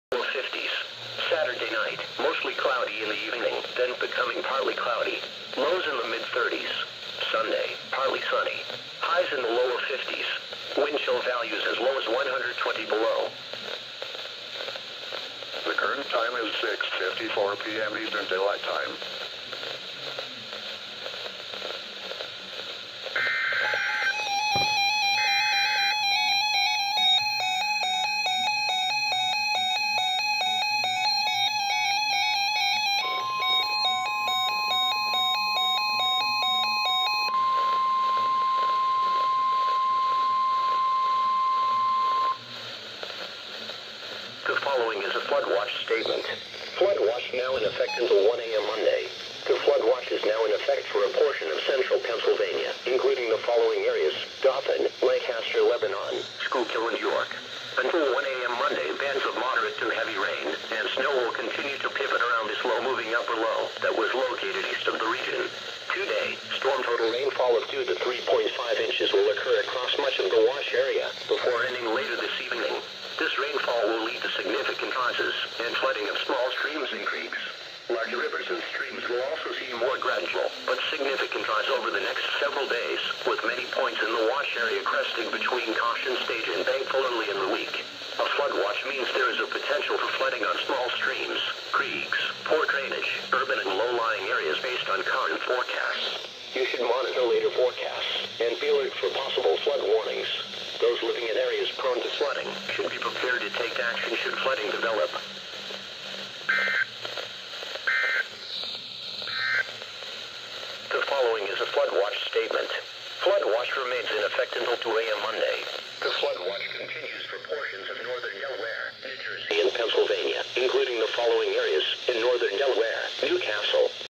{FAIL} EAS #256 - Areal Flood Watch Extension 3-30-2014
Due to static, both the Radio Shacks 12-522 and 12-521 rang early. In addition, the Radio Shack 12-521 read it as a Flood Warning.

Station: WXL39
WFO: State, College, PA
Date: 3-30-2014